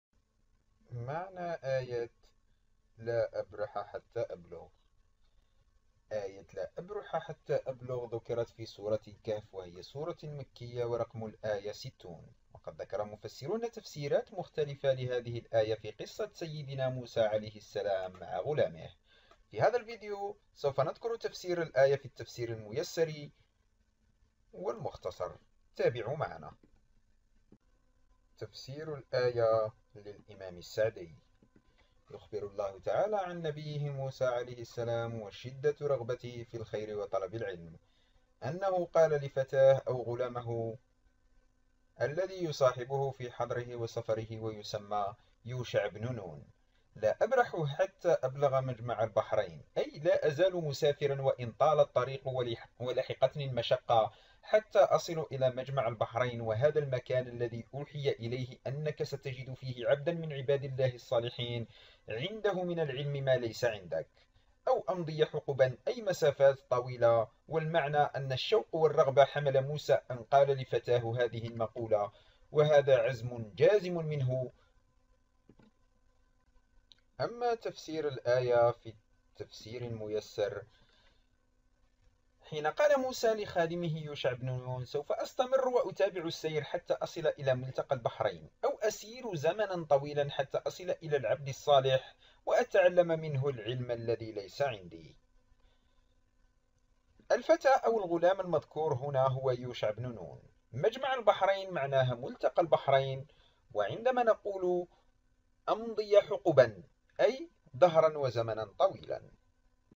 معنى آية : لا أبرح حتى أبلغ
ذكر المفسرون تفسيرات مختلفة لهذه الآية في قصة سيدنا موسى عليه السلام مع غلامه
The commentators mentioned different interpretations of this verse in the story of Moses, peace be upon him, with his servant